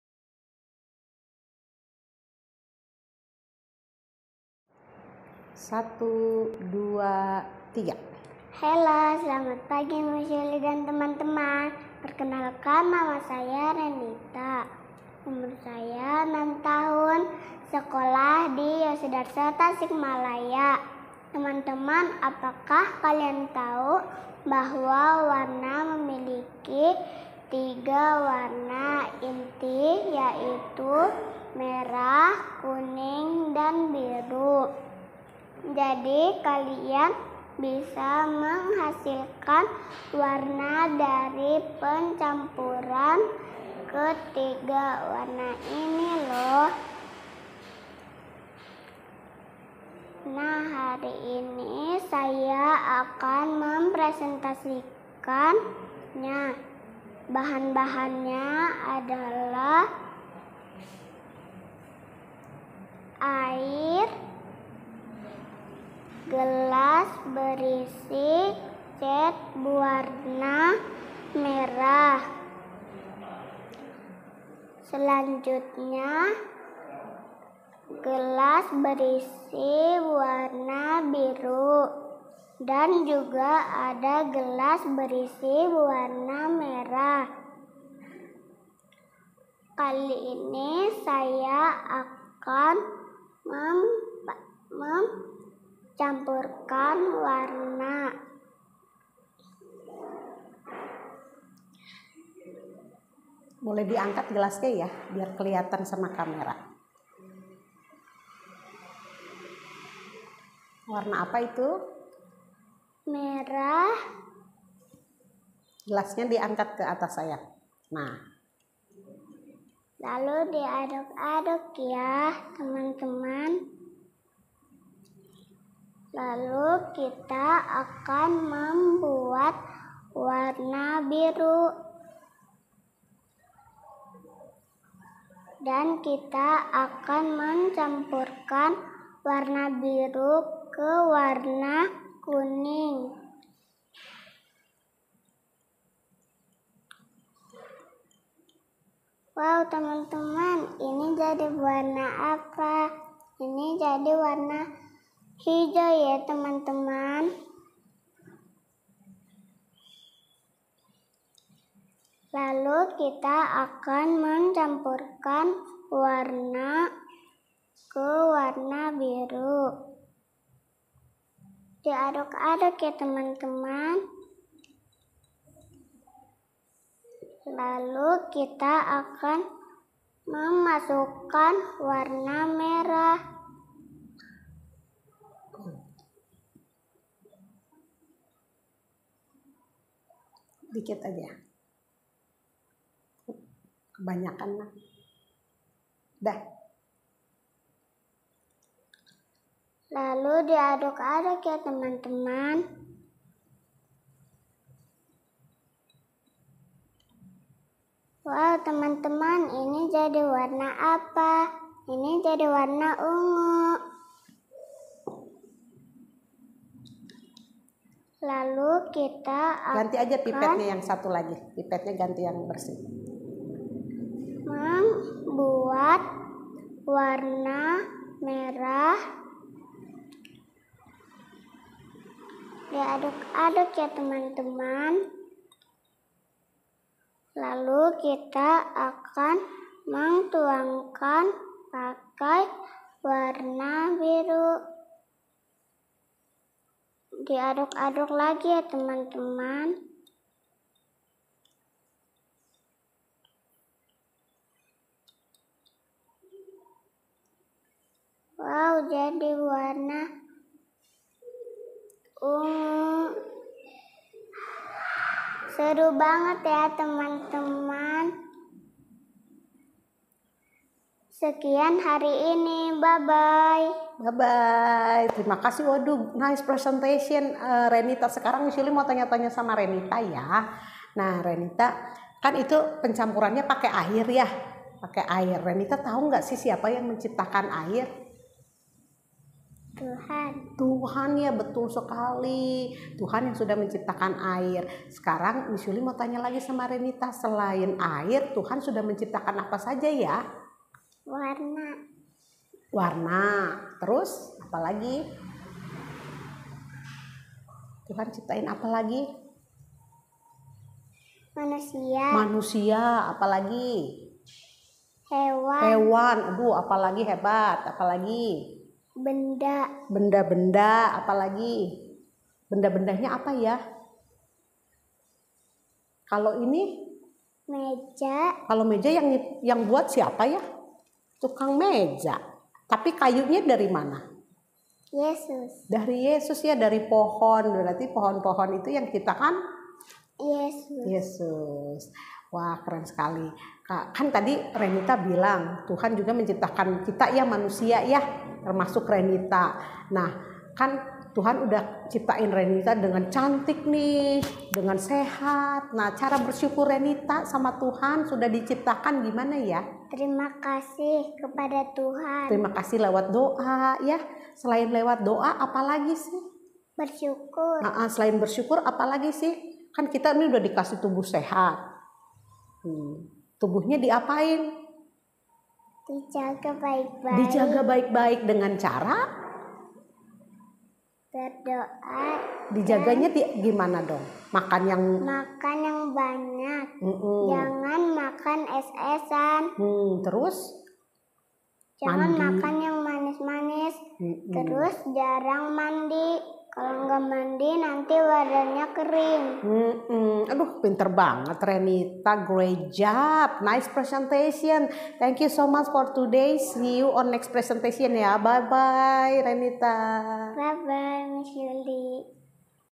Renita Veronica Br Sianturi
Renita kali ini akan mendemonstrasikan pencampuran warna atau mixing color, penasaran warna apa saja yang dibuat Renita yuli kita lihat video presentasinya